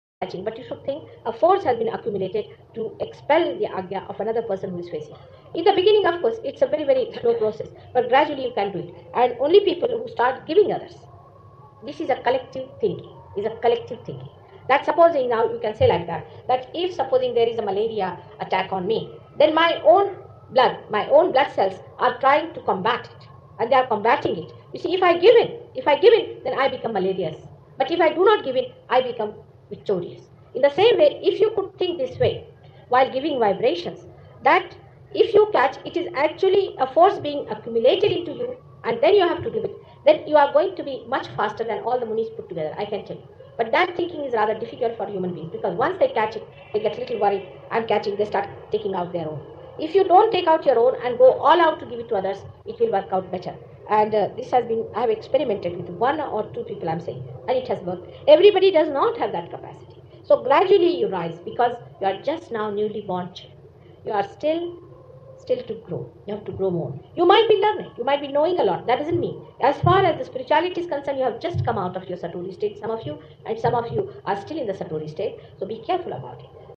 Creation-catches-1977
Shri Mataji Nirmala Devi, on Creation, Delhi, India, 20/02/1977
"In the same way, if you could think this way while giving vibrations: that if you catch it is actually a force being accumulated into you and then you have to give it"